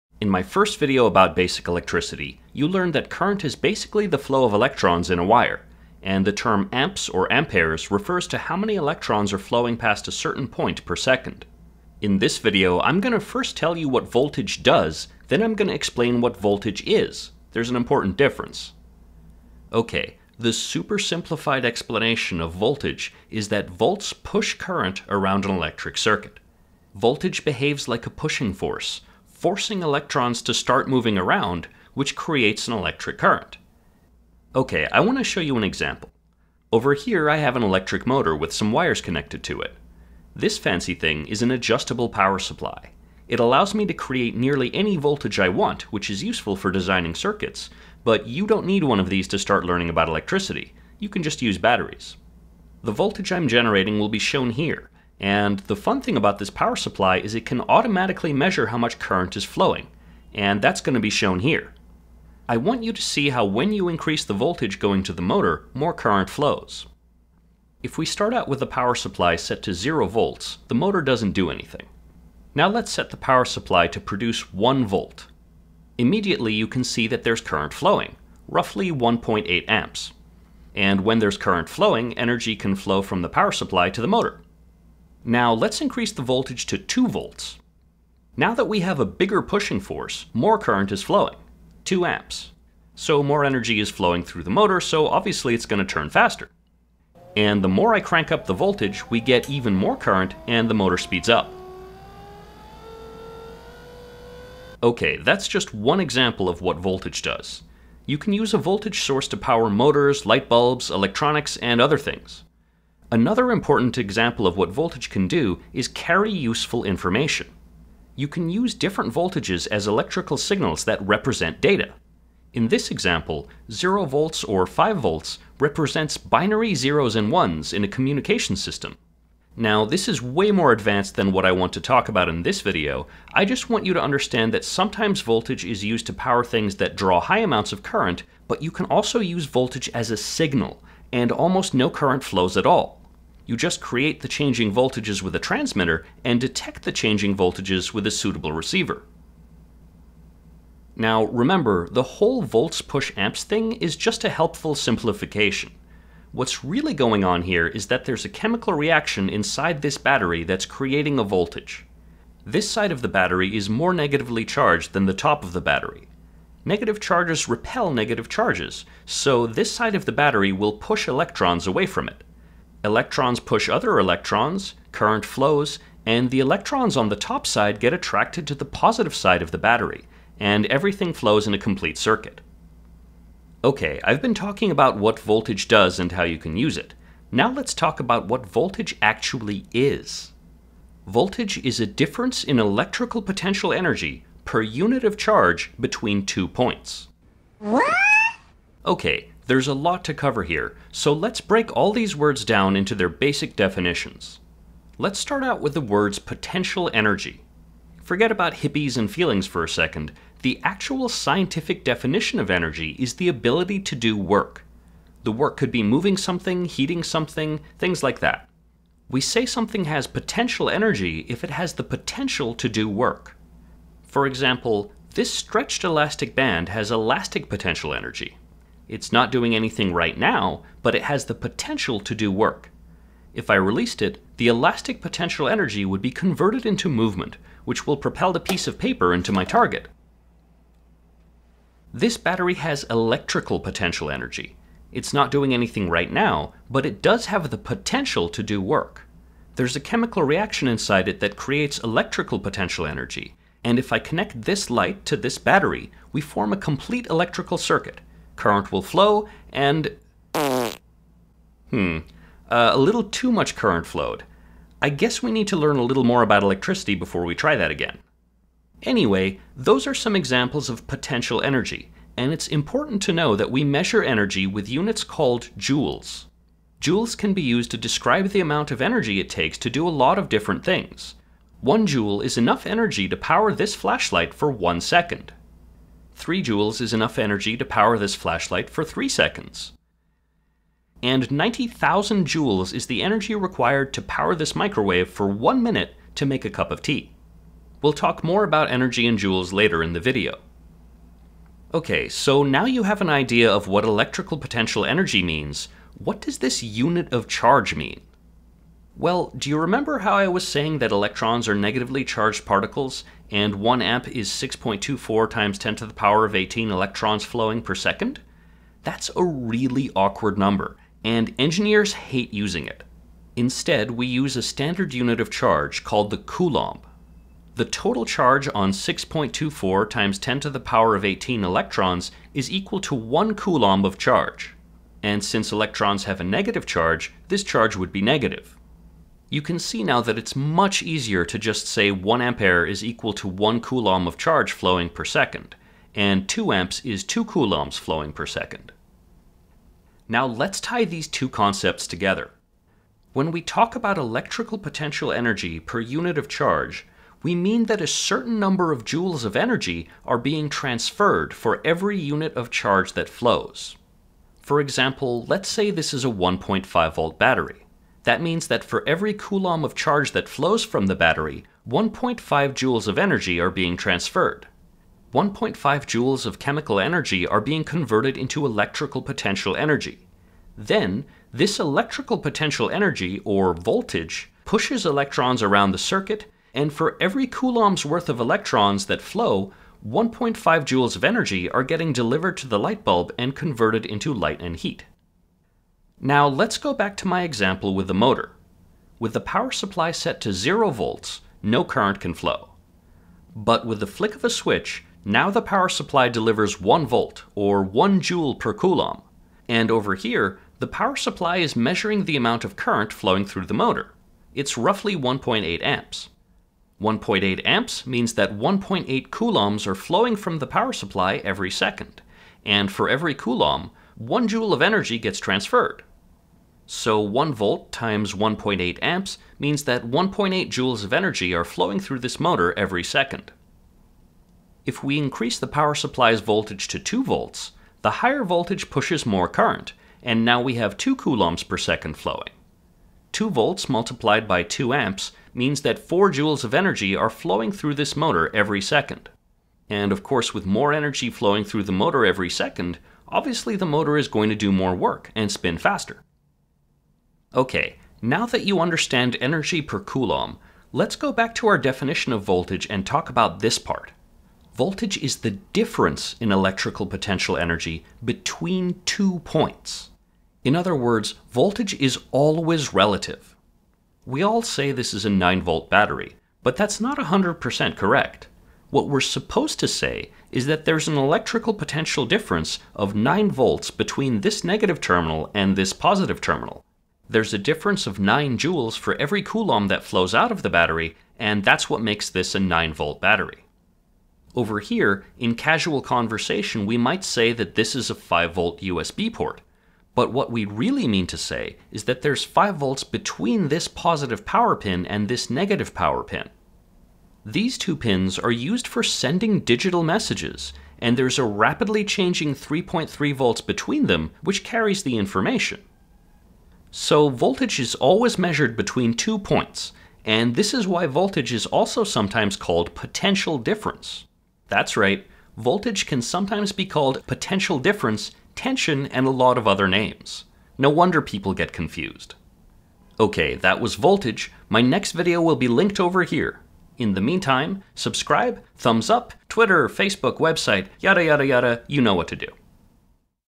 🔌 Basic Electricity - What is voltage?
What is voltage and what does it do? Learn about volts, coulombs, joules, power supplies and more in this video!